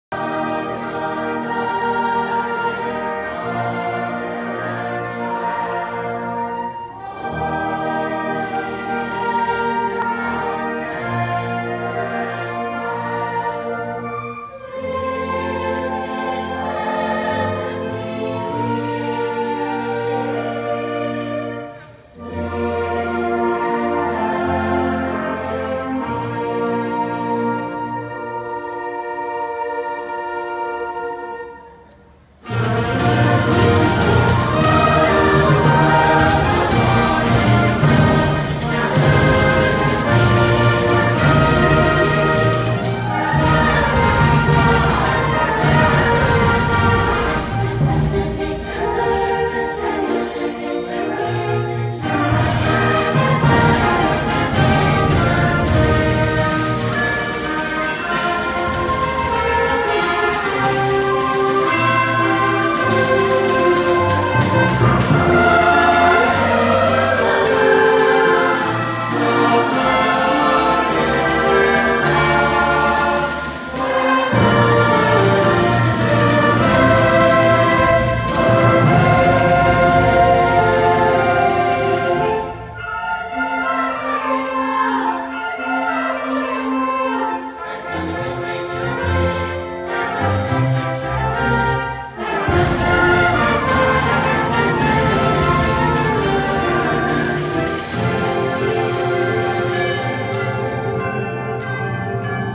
St Francis High School Christmas Concert
Cousins—Georgia and Huba—begin Christmas celebrations with their St Francis High School band members—a beautiful concert celebrating Christ’s birth!